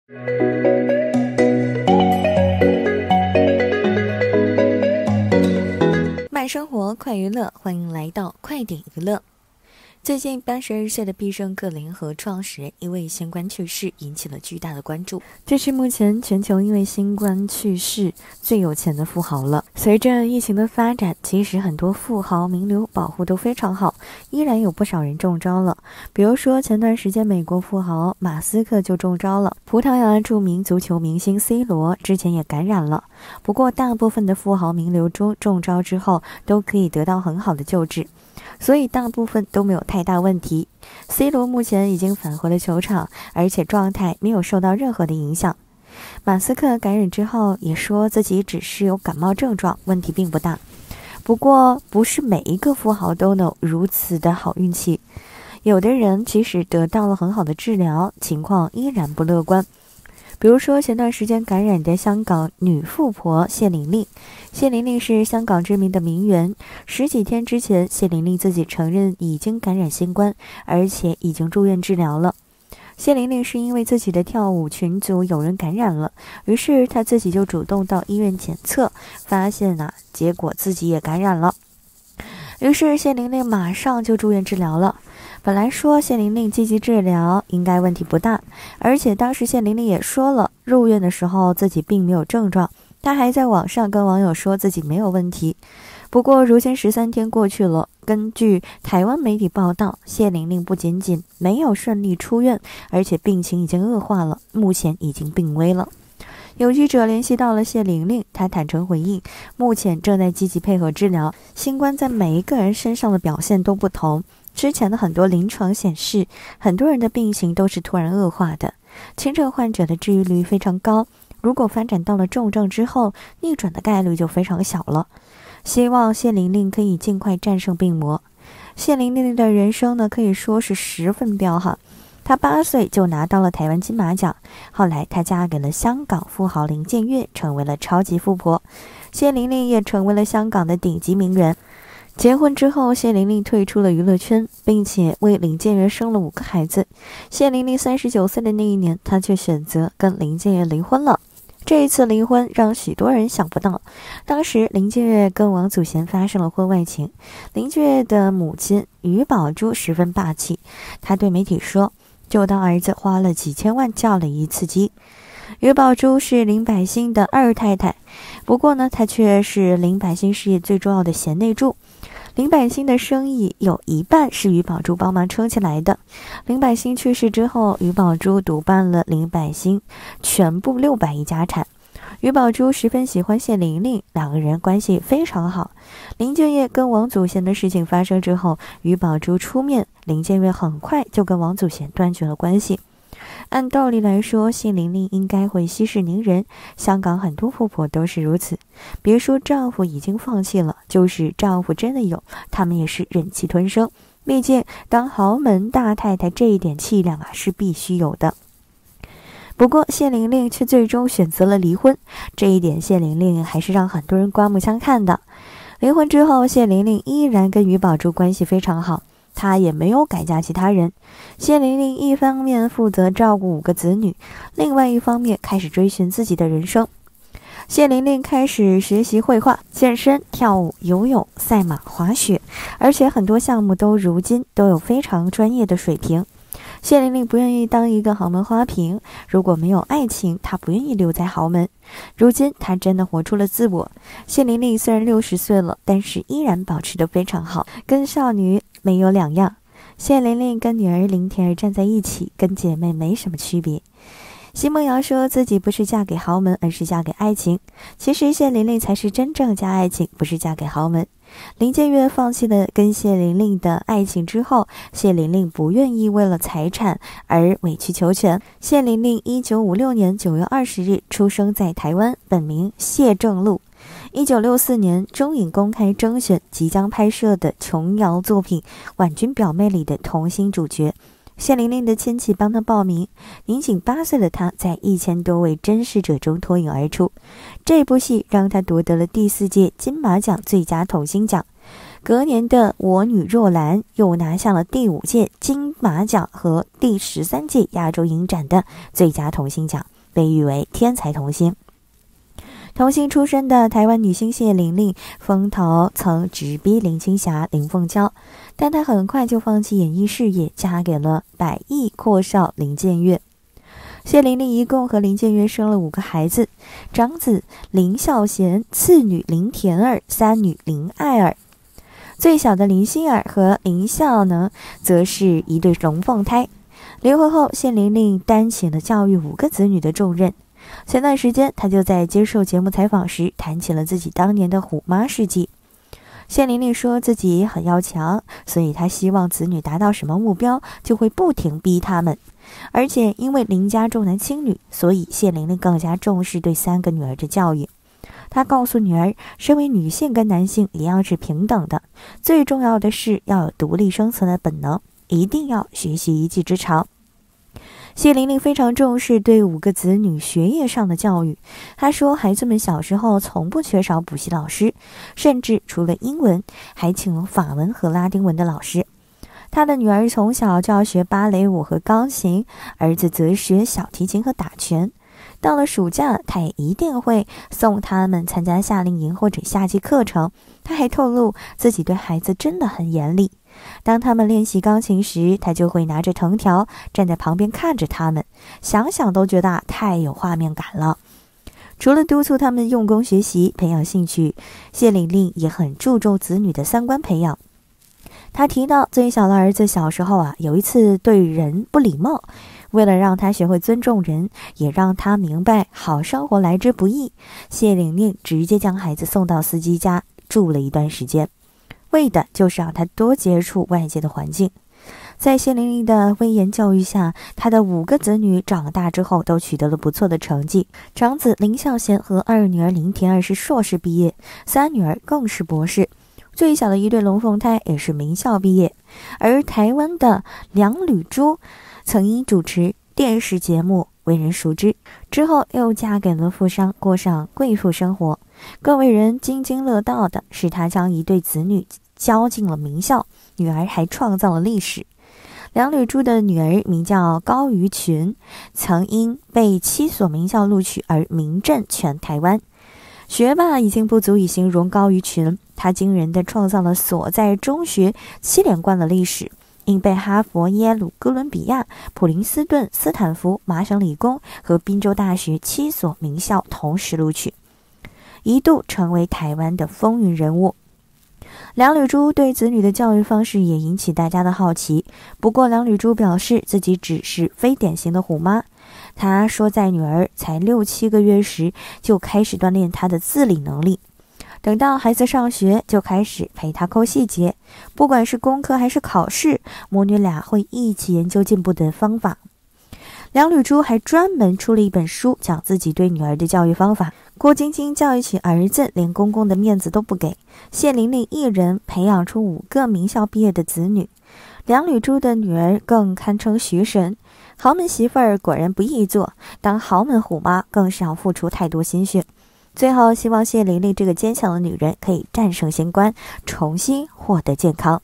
女星嫁給百億富豪，60歲依然年輕像少女，可惜感染新冠如今病危｜快点娱乐
女星嫁給百億富豪，60歲依然年輕像少女，可惜感染新冠如今病危